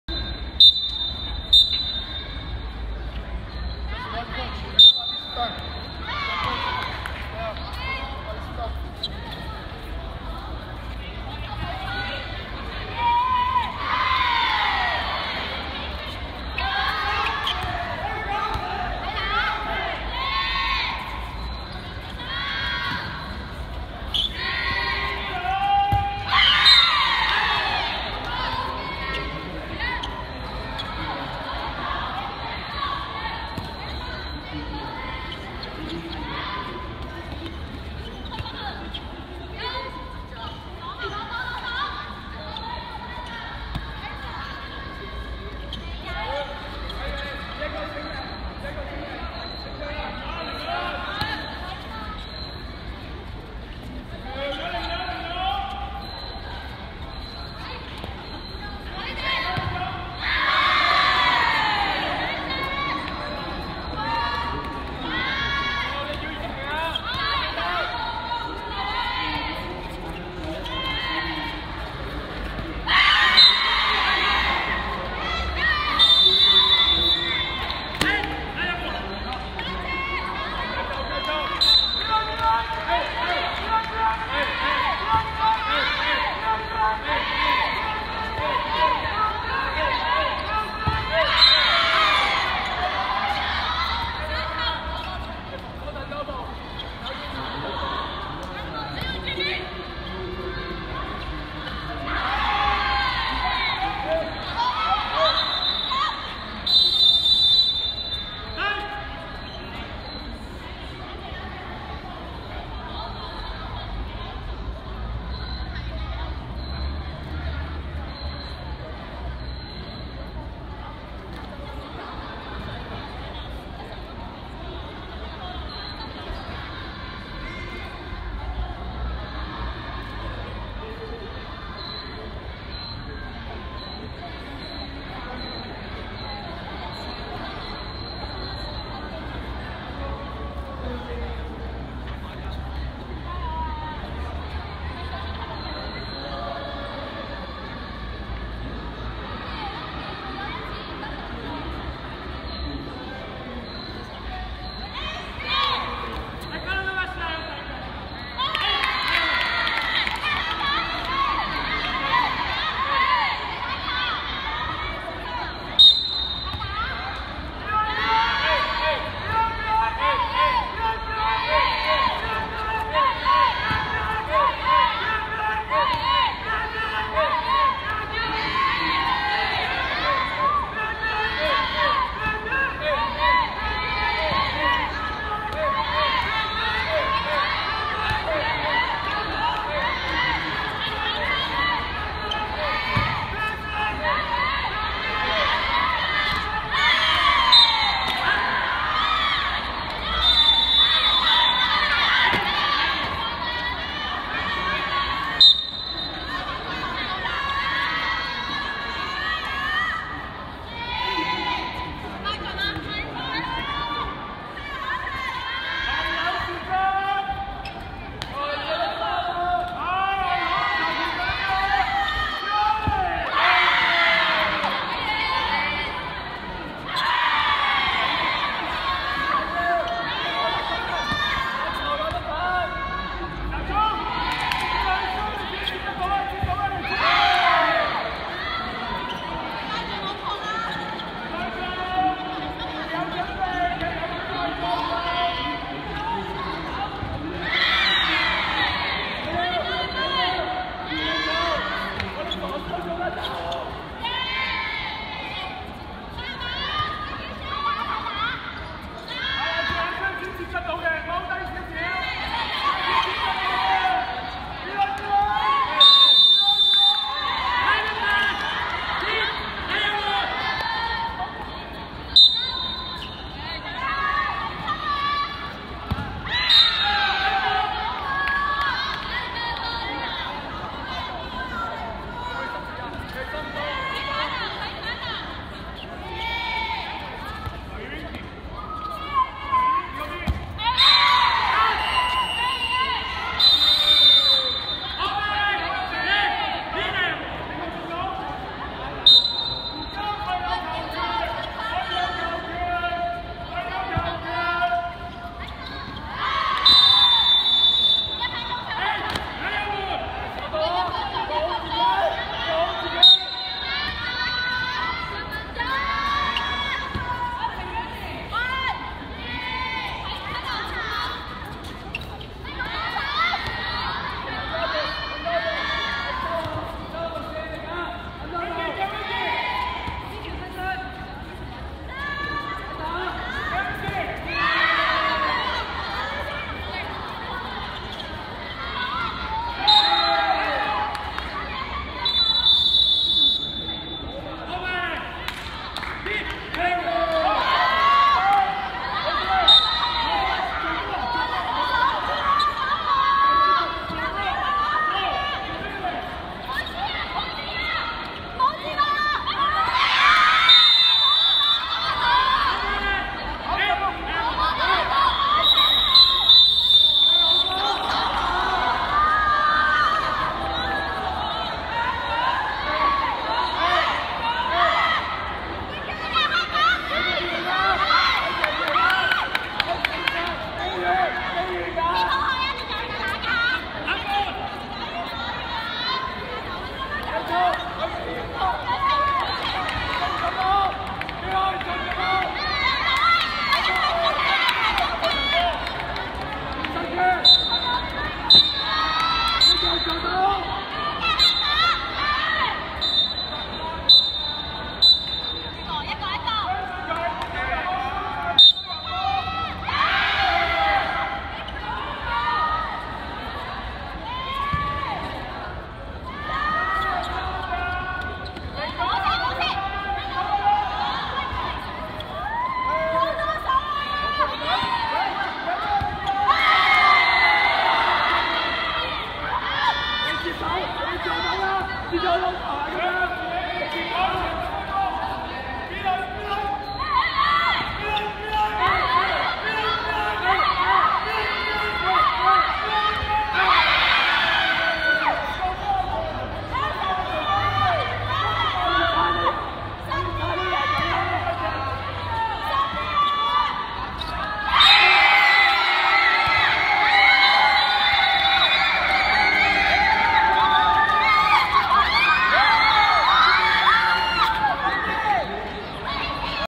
2016 ADC ドッジボールアジアカップ 女子 O13 Hong Kong A vs Hong Kong B (Set 3)
SUPPORT US